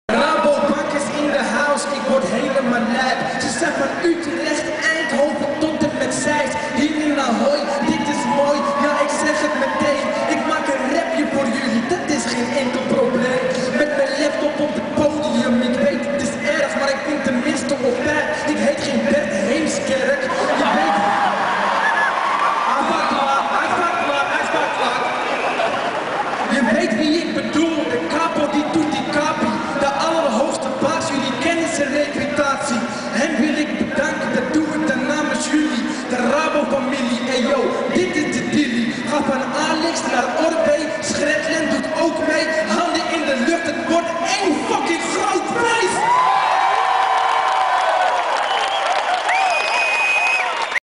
Ali B tijdens personeelsfeest 2007 Rabobank
Ali B met eigen gemaakte rap tijdens personeelsfeest 2007 Rabobank